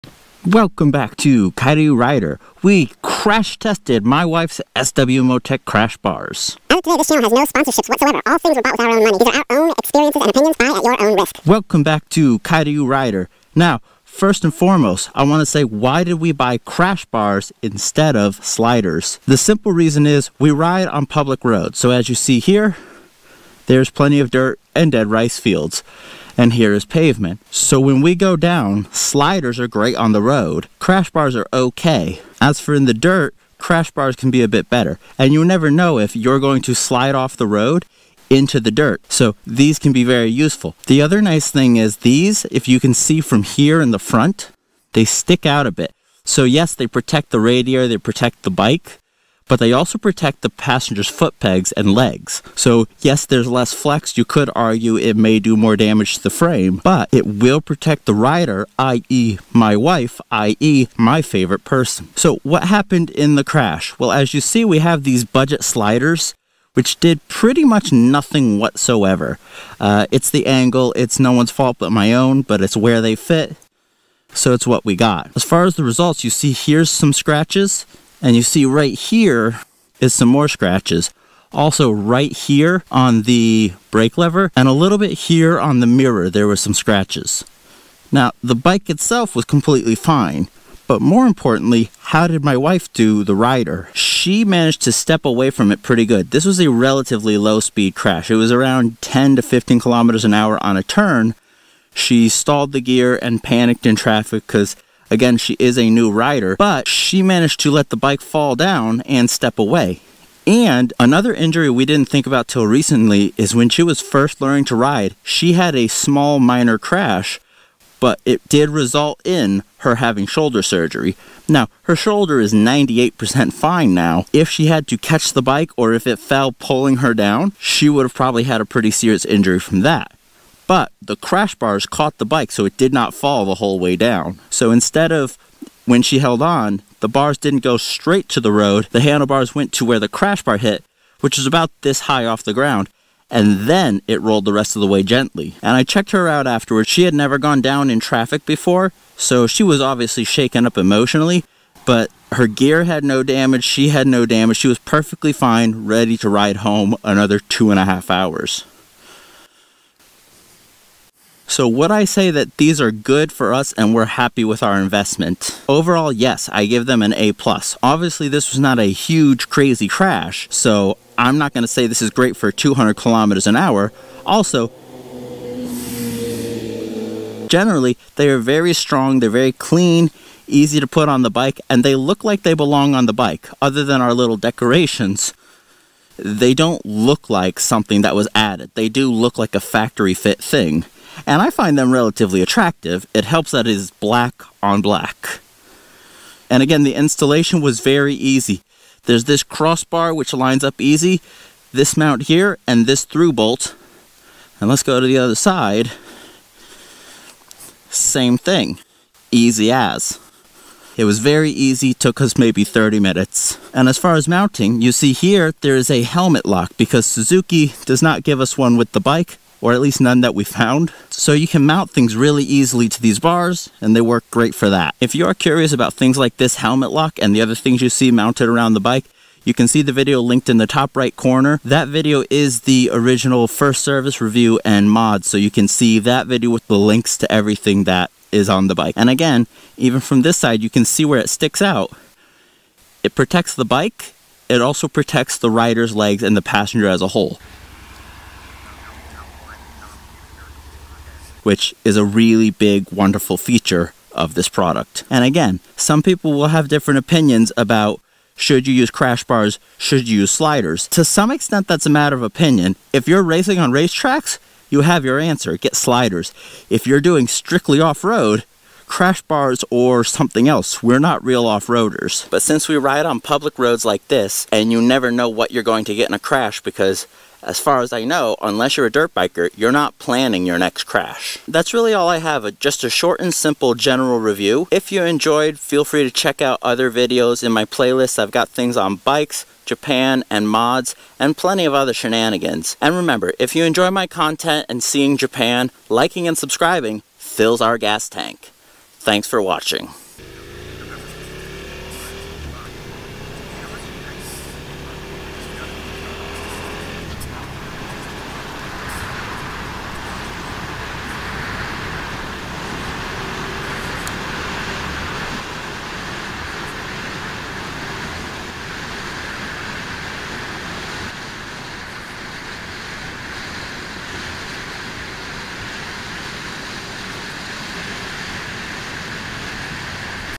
Sw Motech Crash Bars Tested!!!! (SV650)
We accidentally tested the Sw-Motech crash bars for the SV650 this video gets into the results.

I have plenty of other playlists and videos so be sure to look around.